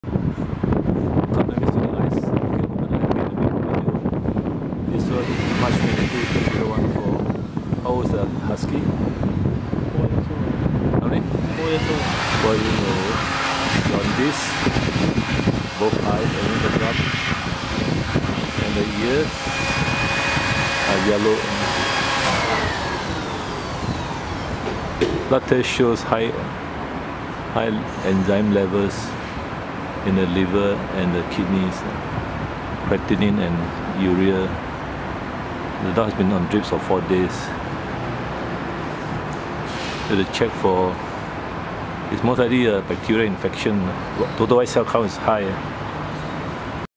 Vomiting in a Husky - liver and kidney disorders
Vomiting for over a week. A blood test is important but the owner must give his consent.  Practising evidence-based medicine means using aids to diagnosis such as blood test and X-ays.

Blood test showed liver and kidney functions are not normal. The dog develops jaundice now. Total white cells are high. Platelets are low.